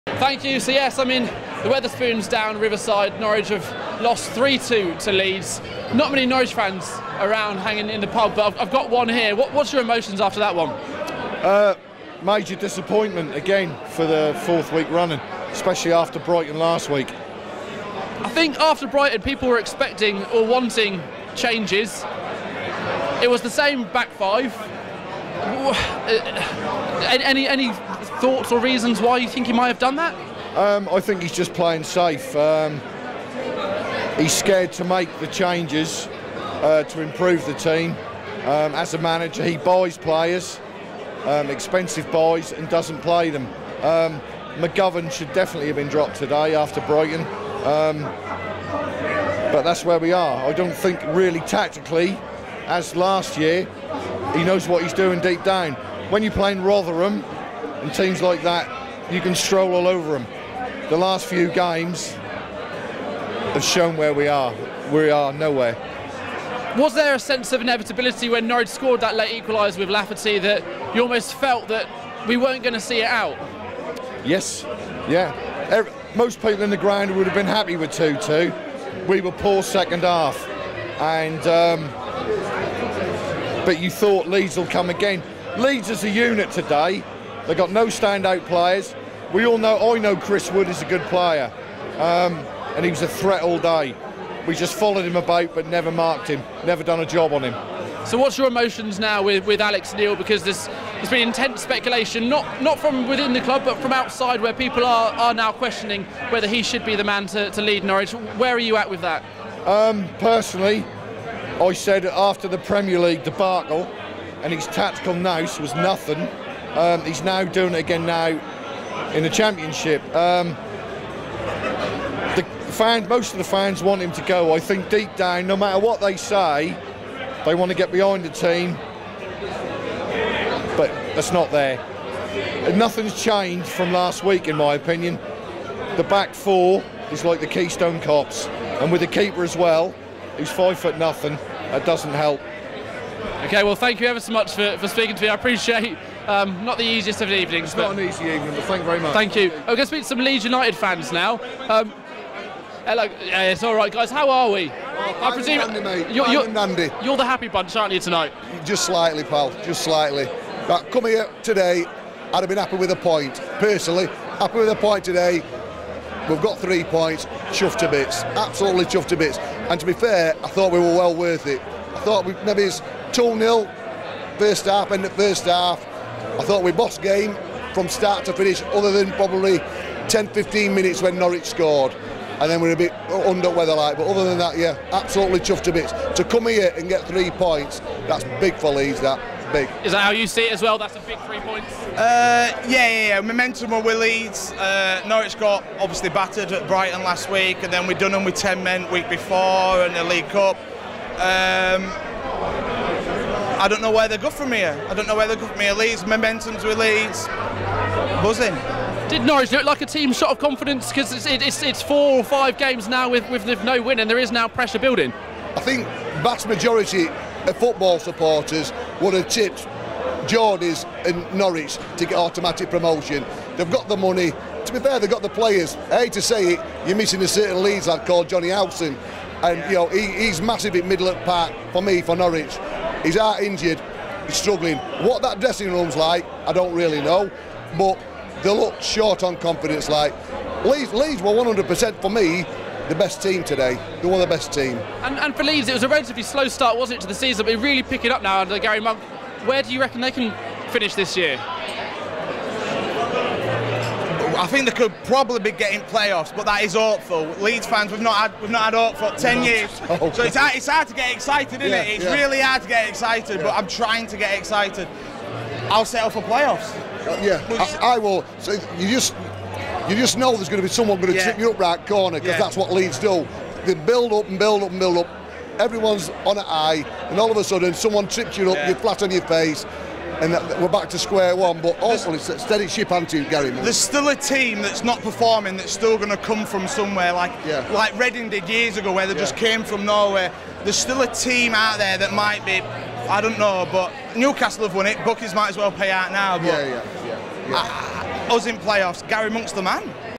Norwich City 2-3 Leeds United: Fans reaction to Leeds' win at Carrow Road
Norwich and Leeds fans down the pub react to the Norwich City 2-3 Leeds scoreline